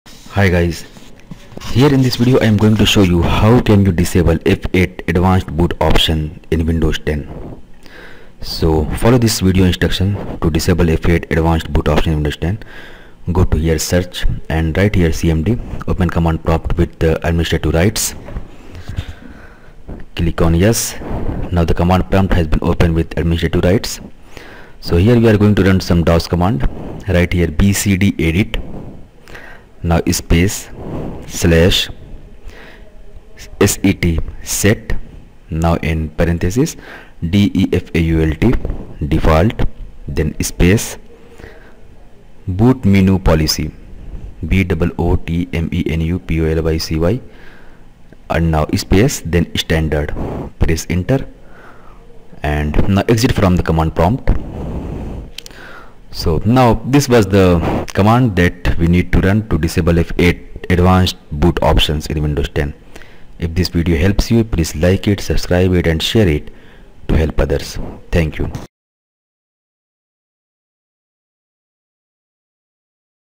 How to disable F8 Advanced Boot Option in Windows10
This video will show you how can you disable F8 key advanced boot option in windows 10, just you need a dos command to disable this option, watch this video to know that command and how to run that command.